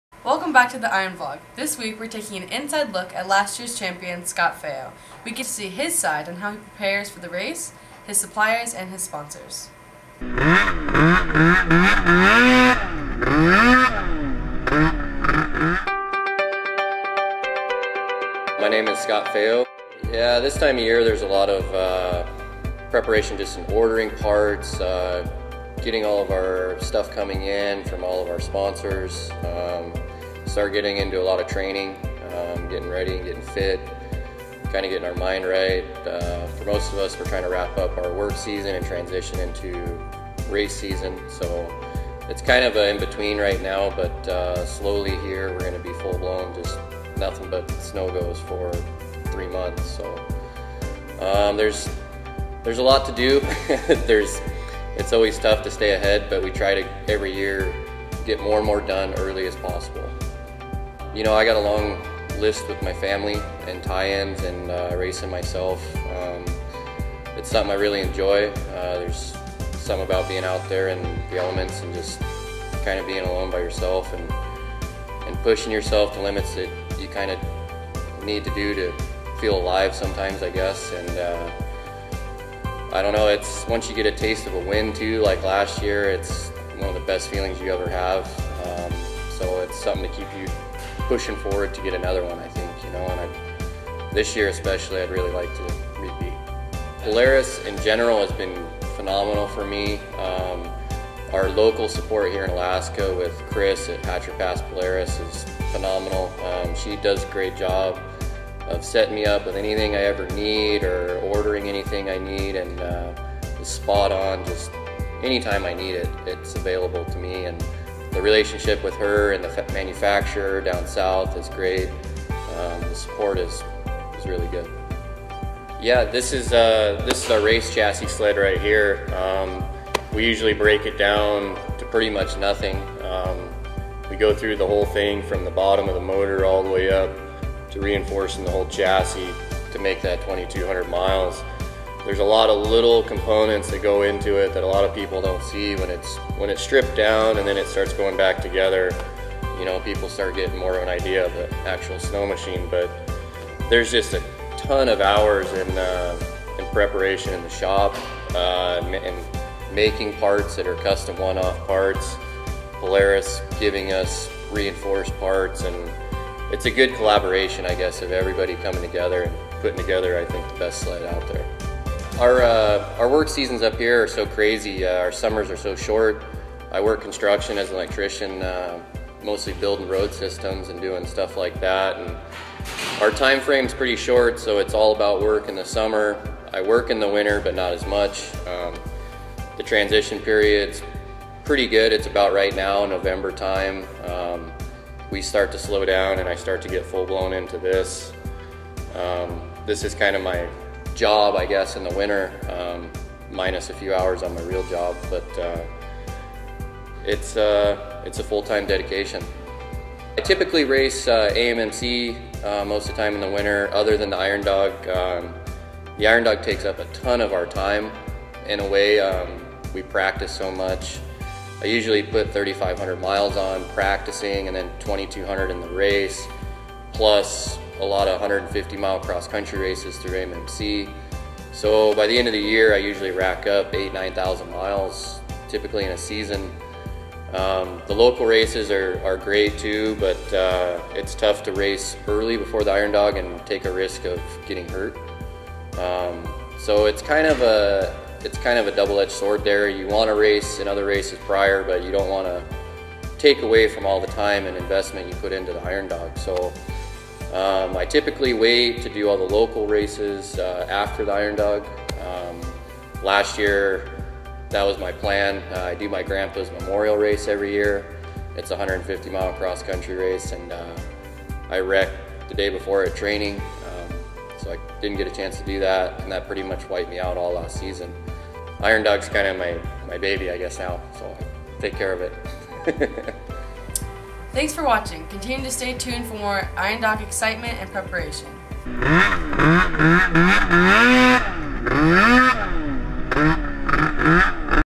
Iron Vlog Episode 3: Scott Faeo Pre-season
Join SAHS student Sophie as she interviews top racer, Scott Faeo! Learn about his process and way of life leading up to the world's longest and toughest snowmobile race, The Iron Dog.